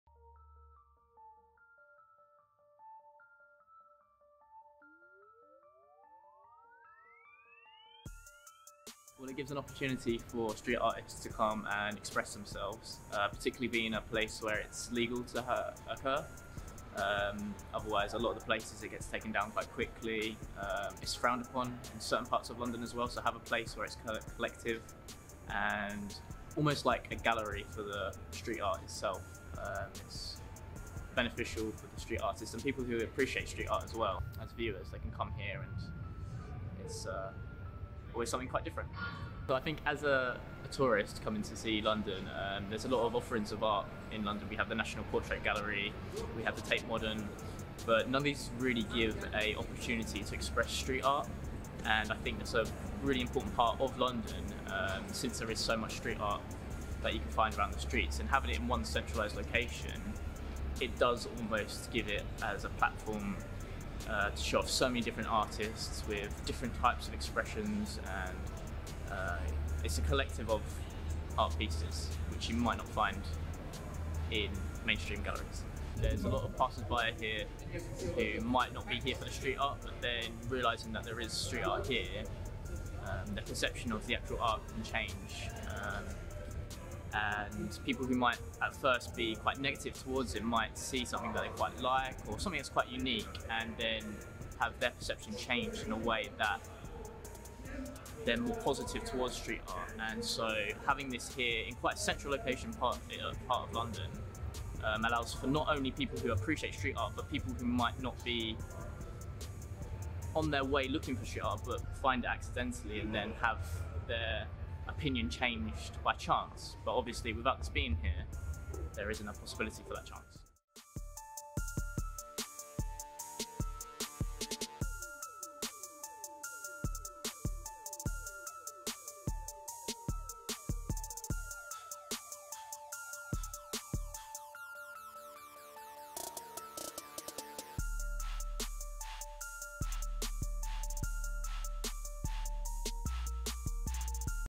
LEAKE STREET- Unique London
Check out the first of a five part series showcasing unique areas of London and what they bring to the city. In this video we will be looking at Leake Street, located in Waterloo. Leake Street offers passers-by with an artistic enviornment that is forever changing. The tunnel is a free to go area that enables street artists to freely spray on the wall with no legal complications! Make sure you come on down and see this amazing place!

Make sure you check our Leake streets social media's too!

[Non profitable] [University project]

Directed by Michael Hopkins
Produced by Izzy Foote
Camera work by Louis Podmore